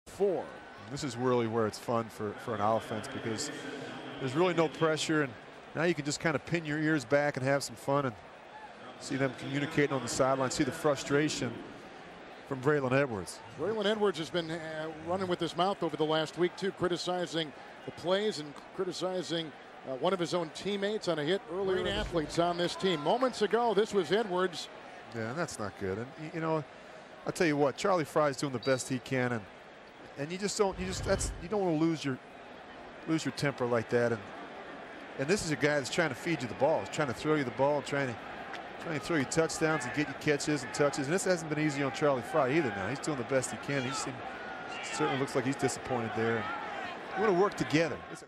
Charlie Frye-Braylon Edwards Sideline Altercation (2006)
I used a snippet of this in my 2007 Browns documentary.